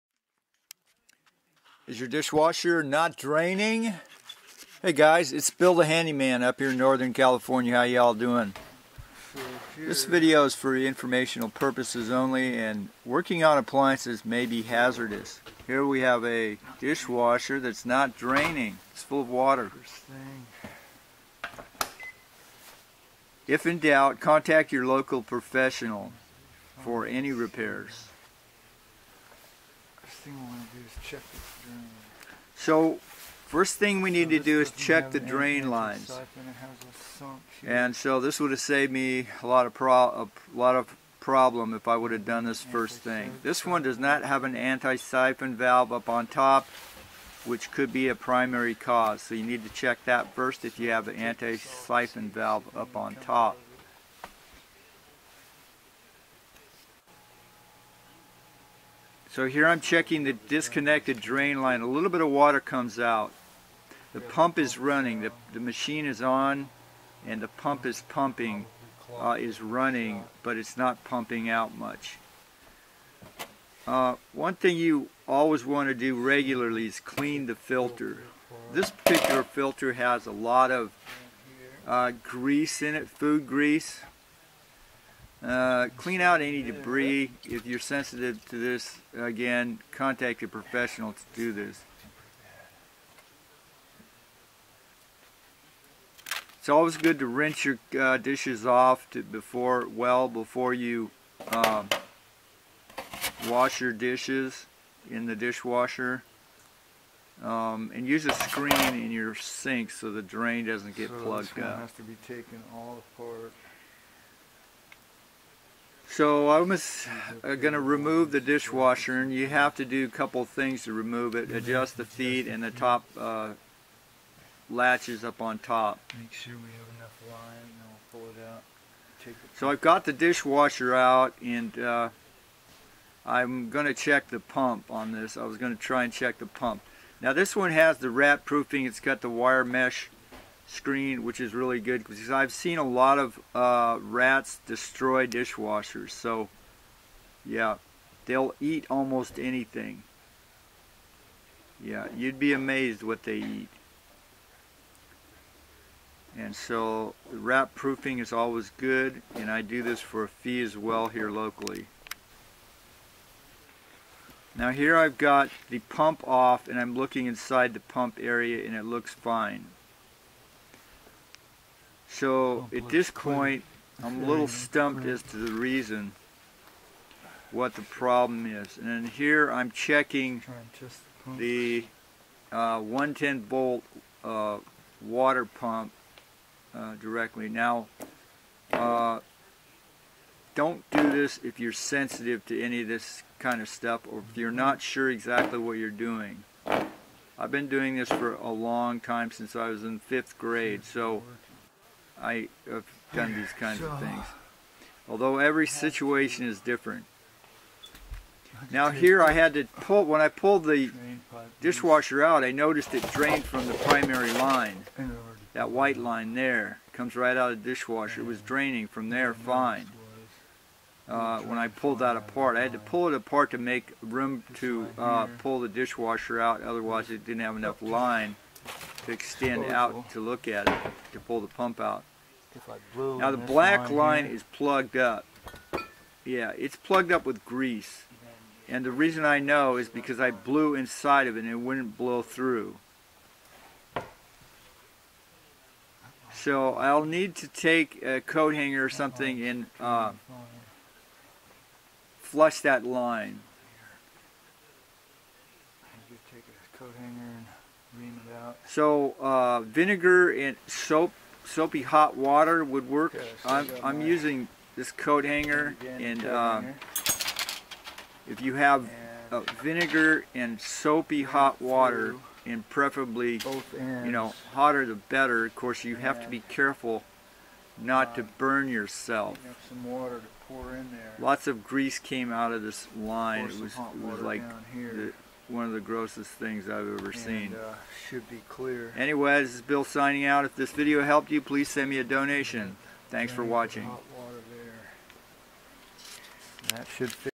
Your dishwasher not draining?
Check your drain lines first. Bill's Entrps. z_fixitman @ yahoo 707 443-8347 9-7 I give advice for a fee. Also do service locally. Thank you for reusing, recycling.